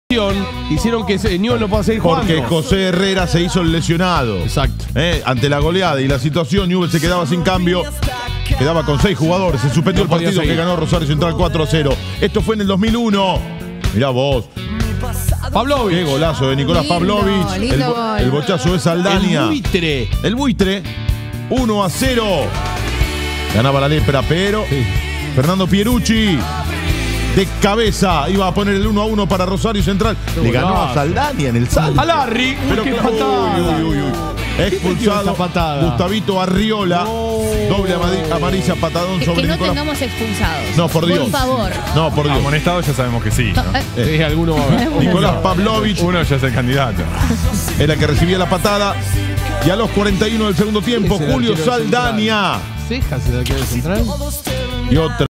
Clausura 2001 | R. Central 1 - Newell's Old Boys 2
Fecha 17 = Domingo 27/05/2001
R Central   1 - 2  Newell's Old Boys

+ R Central: Laureano Tombolini; Javier Cappelletti, Ricardo Canals, David Pérez, Leandro Diema; Marcelo Quinteros, Daniel Quinteros, Pablo Sánchez; Gustavo Arriola; Federico Arias y Fernando Pierucci. Director técnico: Edgardo Bauza.

+ Newell's Old Boys: Sebastián Cejas; Raúl Damiani, Fabricio Fuentes, Fernando Crosa, Valentín Filipini; Julio César Saldaña, Juan Sánchez, Maxi Rodríguez; Damián Manso; Mauro Rosales y Nicolás Pavlovich. Director técnico: Jorge Ribolzi.

Goles: PT 17' Pavlovich (N) y 19' Pierucci (R). ST 41' Saldaña (N).
Cambios: PT 40' Juan Pablo Vojvoda por Fuentes (N). ST 20' Liber Vespa por Marcelo Quinteros (R); 27' Germán Real por Damiani (N), 31' Javier García por Arias (R) y 45' Diego Bono por Rosales (N).
Expulsados: ST 21' Arriola (R) y 45' Cappelletti (R), por doble amonestación y la segunda por juego brusco.
Arbitro: Daniel Giménez.
Cancha: R Central.
Hora: 15:30